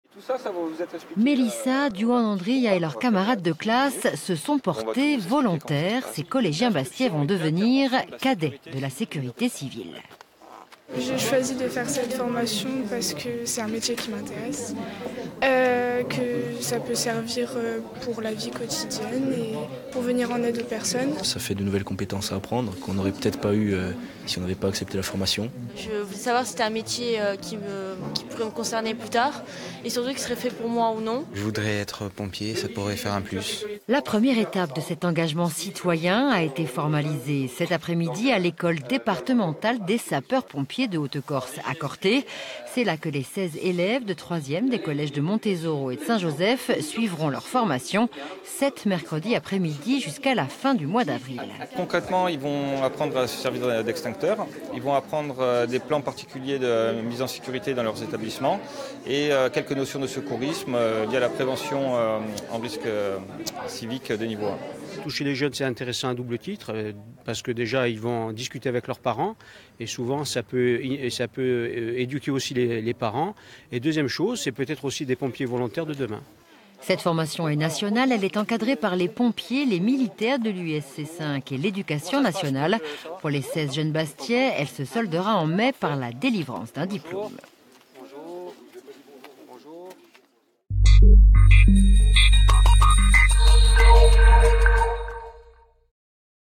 Corte - 16 nouveaux cadets de la Sécurité civile
16 adolescents intègrent les cadets de la Sécurité Civile de Haute Corse. Ces collégiens suivront sept demi-journées d'enseignement pour acquérir des compétences en matière de secours. La formation est destinée aussi à développer leur sens civique.